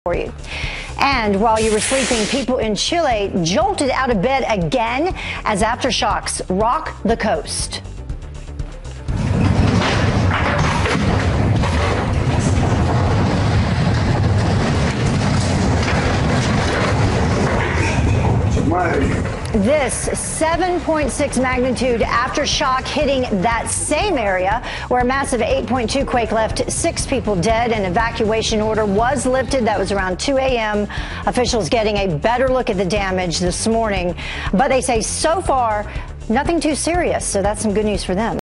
Earthquake : 7.6 Aftershock strikes same area off the Northern Coast of Chile (Apr 03, 2014)
News Articles:

7.6-magnitude aftershock strikes off coast of Chile, USGS says

7.6 aftershock hits same area of northern Chile as Tuesday's 8.2 quake

Chile hit by powerful quake aftershock, no major damage